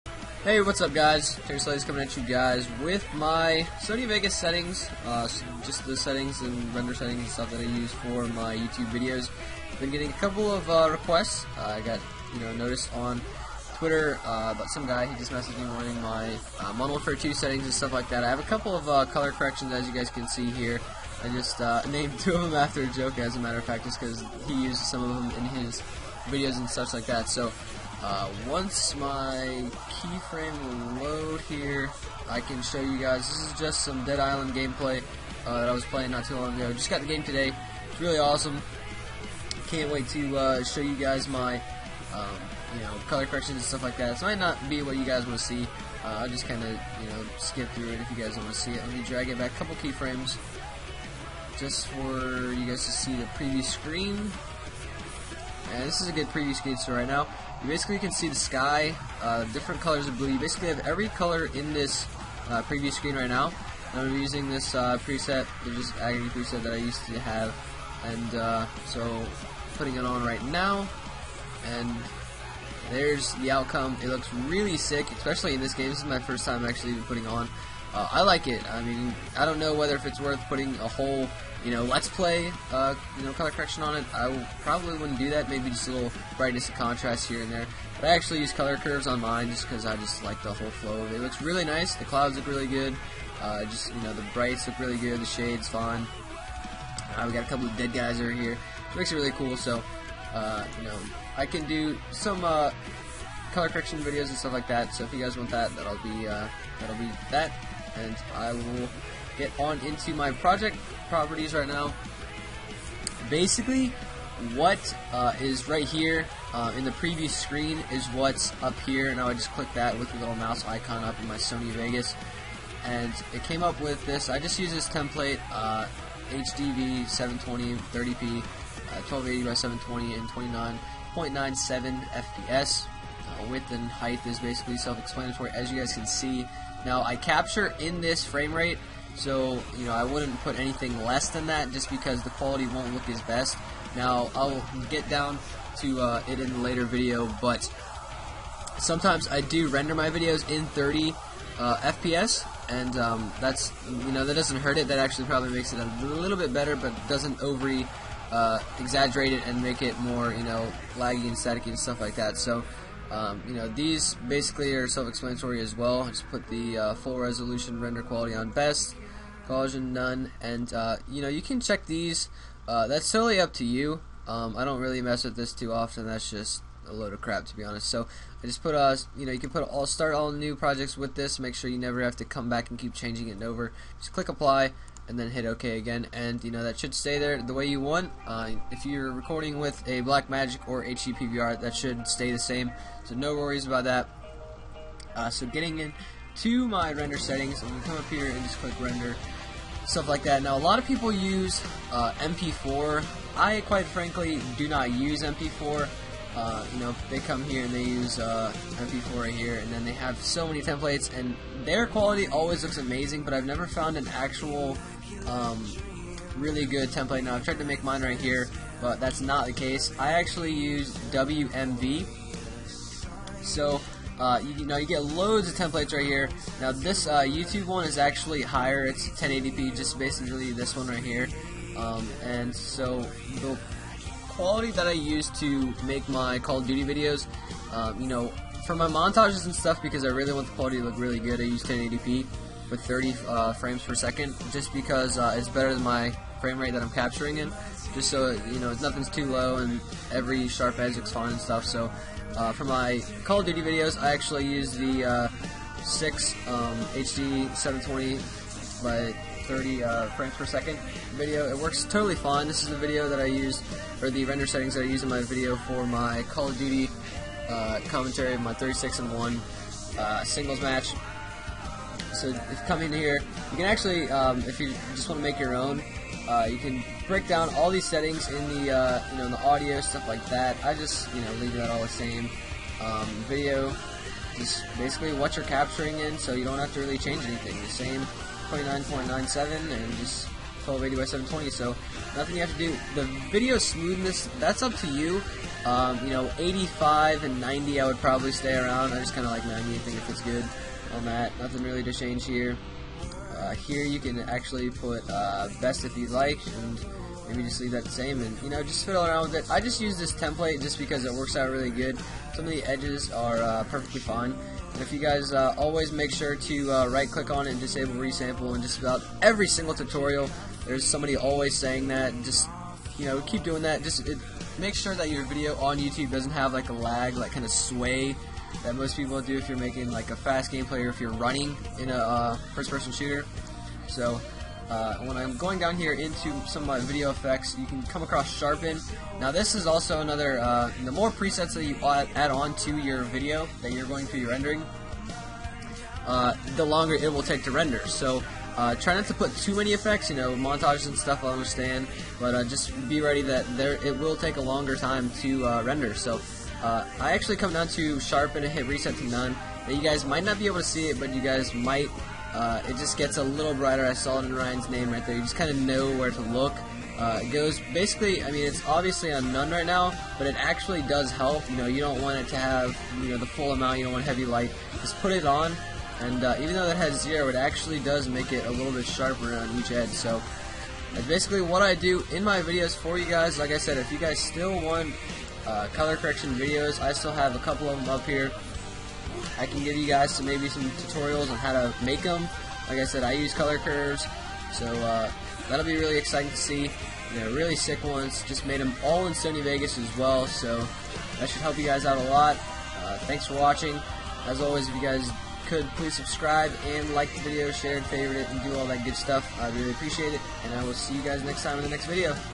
Sony Vegas Tutorial - Project and Render Settings
I've gotten quite a few messages about my render settings and settings in general on my videos. I found the best screen recorder and put this together. These are the settings I use for my Call of Duty videos. Any questions you have about the tutorial I can answer in the comment section below!